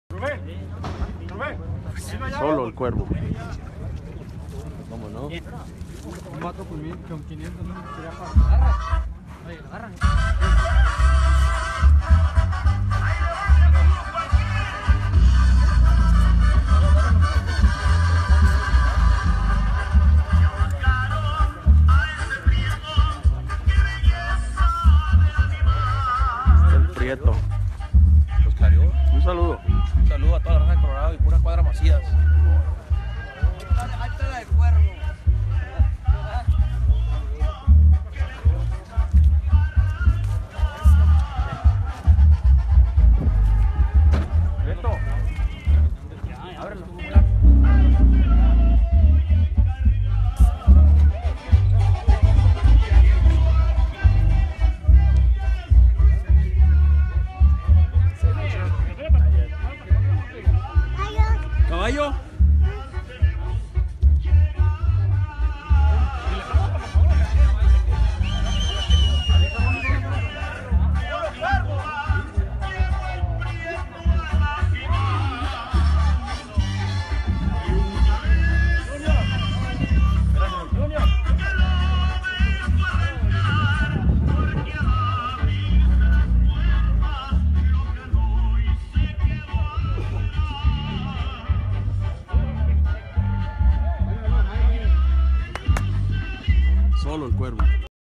El Cuervo Cuadra Macias
EL CUERVO CUADRA MACIAS DESPUES DE GANAR CAMPIONATO EN EL 2 SUERTES